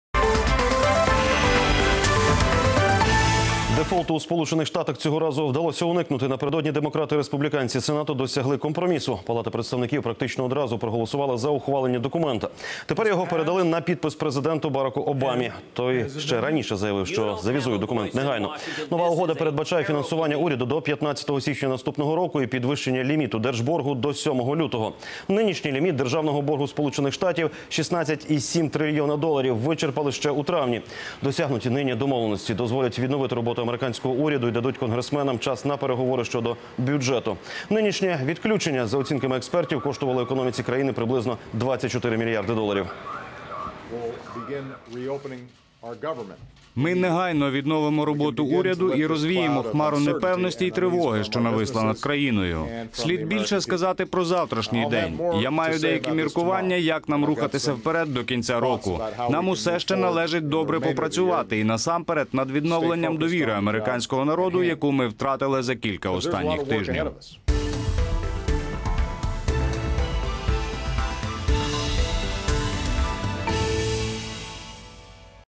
Обама отсрочил дефолт США на 4 месяца
Накануне демократы и республиканцы Сената достигли компромисса

Напередодні демократи й республіканці Сенату досягли компромісу